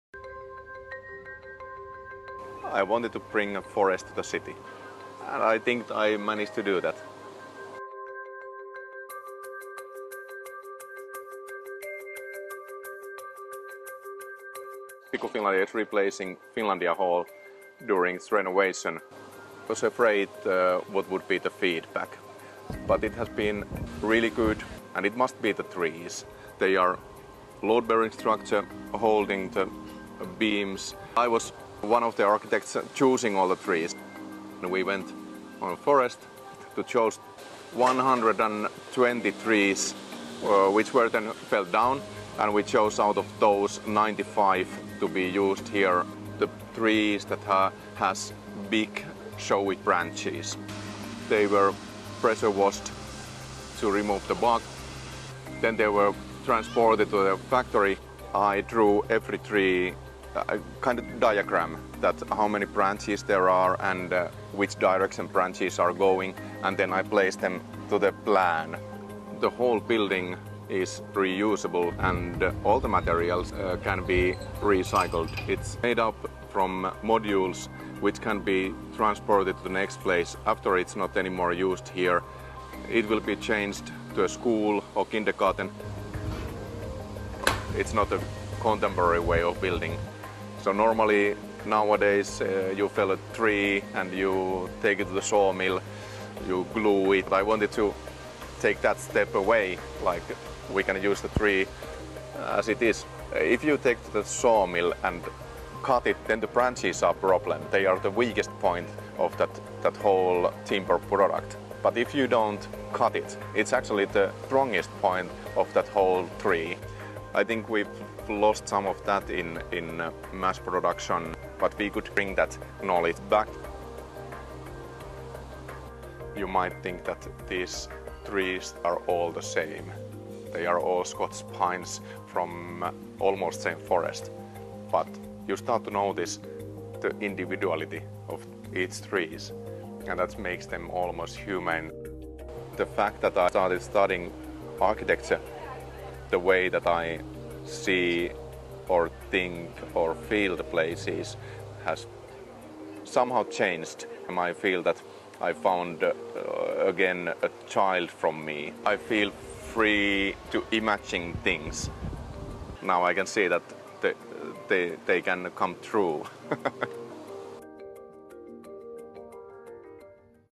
A NEW BUILDING SUPPORTED BY TREES
It’s a reusable wooden building that includes whole trees with protruding branches as part of the structure. Fully recyclable and transportable, it will be packed up and taken to a new location to be used as a school or a nursery in 2024.